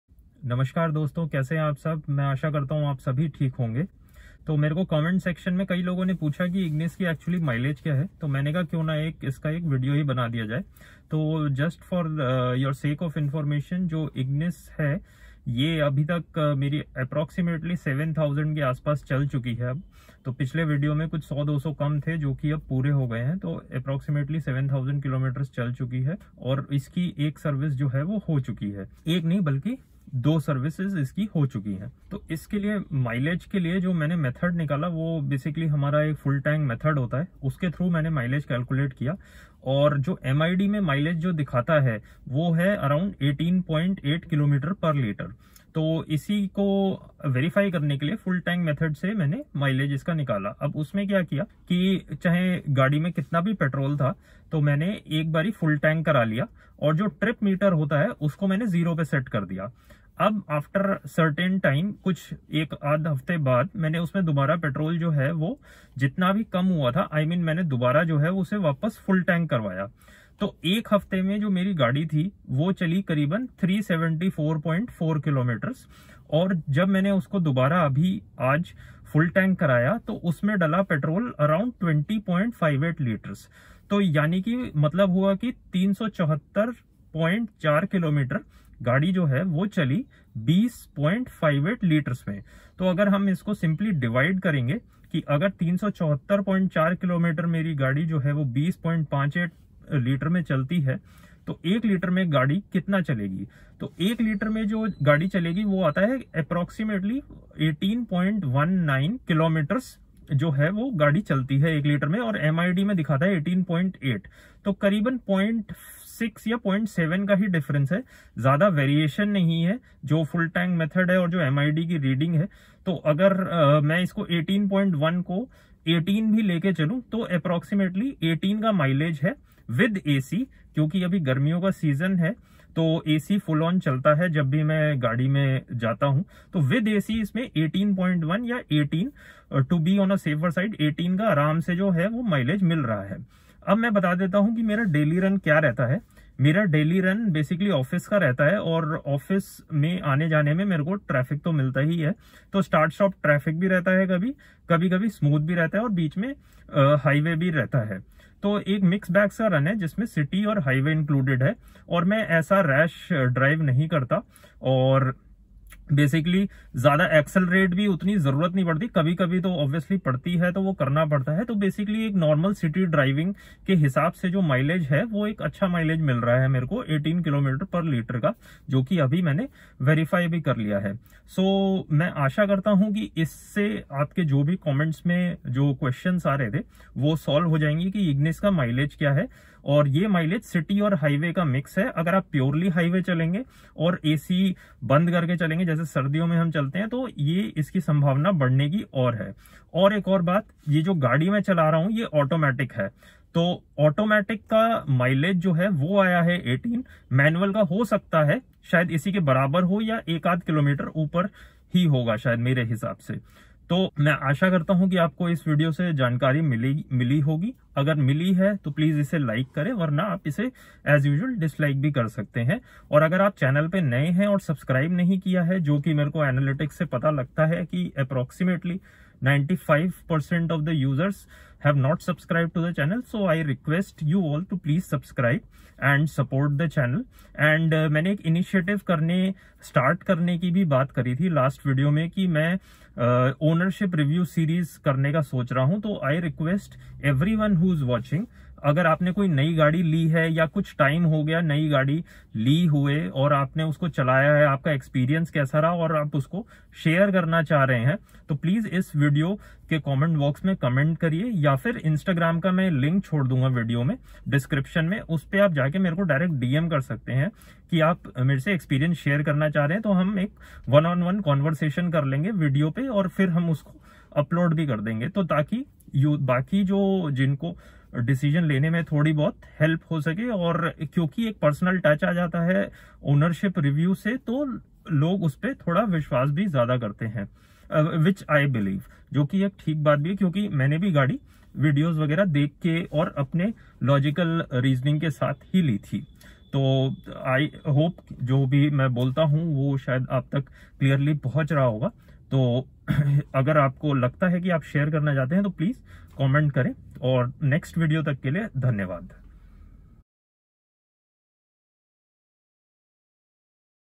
Ignis Petrol Mileage after 7000km | Hindi | 2020
In this video, I tell you about Ignis Petrol Mileage. I have calculated mileage by full tank method & compared with the MID reading.

My other video links for your reference

My Vlogs -